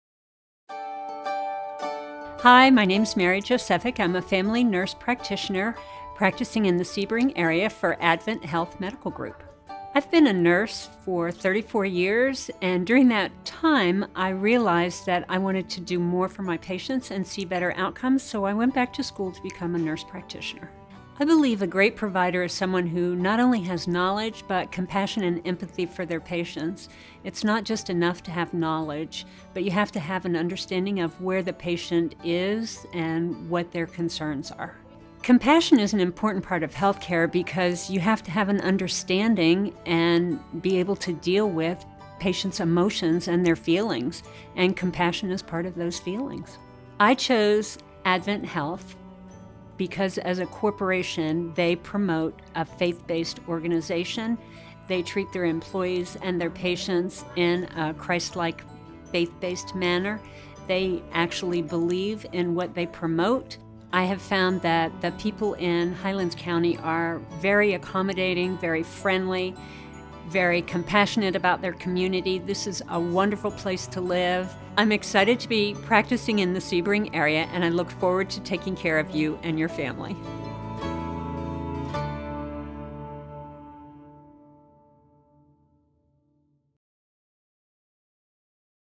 Meet Mary Josefyk, APRN
Mary Josefyk, APRN provides family medicine care in Sebring. Meet her in advance by watching this informative video.